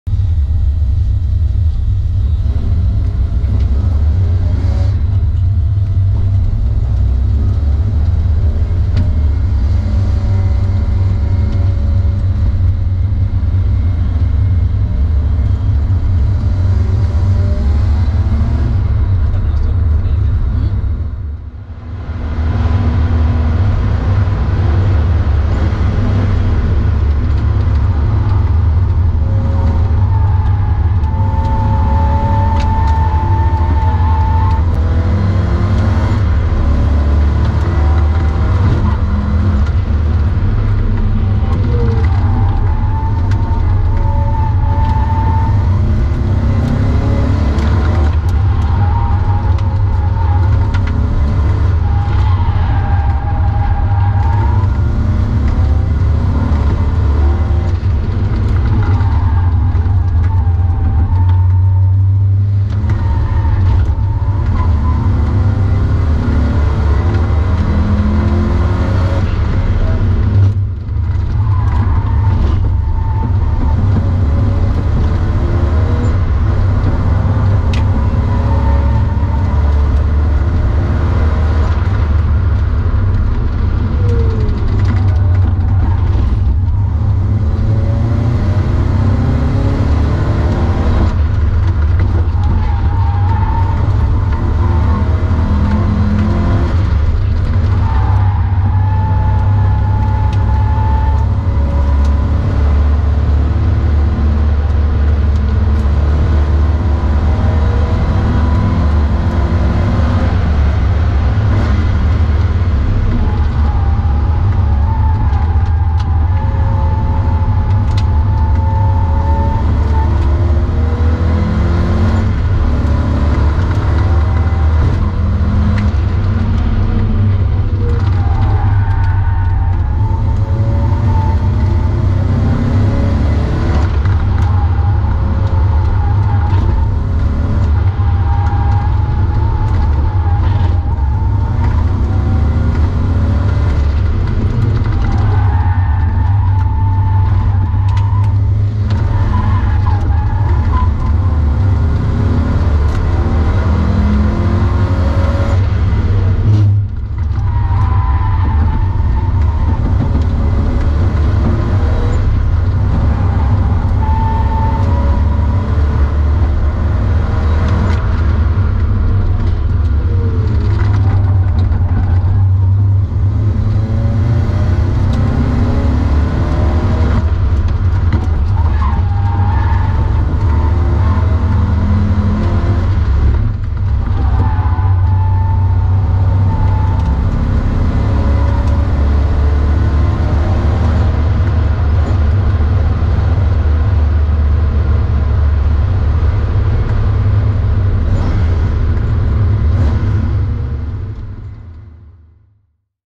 Alfa Romeo Alfasud 1,5 TI Jyllandsringen 23.06.17
Taking the fun Alfasud 1,5 TI out for a spin on FDM Jyllandsringen - Denmarks biggest racetrack.

Unfortunately the GoPro was set up wrong, which overexposed the image.